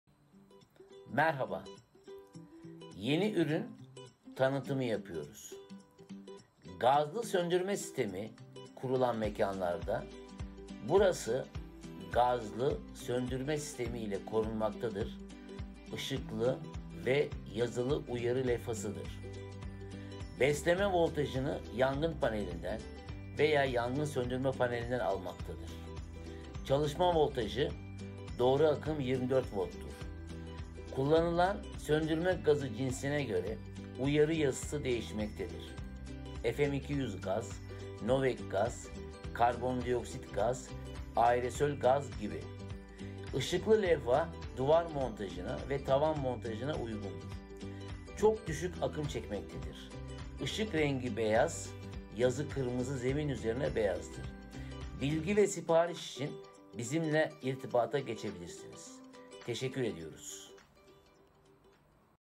IŞIKLI ve YAZILI İKAZ LEVHASI
Gazlı Yangın Söndürme Uygulaması . Yapılan Mekanlarda , Dışarıdan Görülecek Şekilde IŞIKLI ve YAZILI UYARI LEVHASI Konulması Faydalıdır. ÜRETİCİ Firma Olarak Bu İhtiyaca Yönelik Üretim Yaptık.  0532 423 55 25